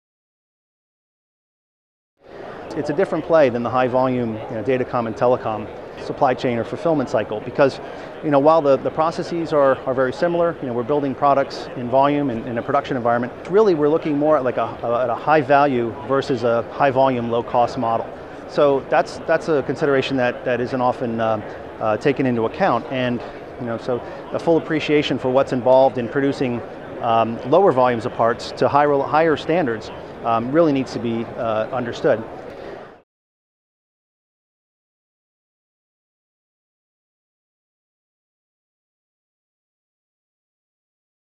Specifying High Quality, Low Volume Photonic Devices
G&H's Frank Weiss talks about purchasing high quality, low volume photonic devices for rugged environments.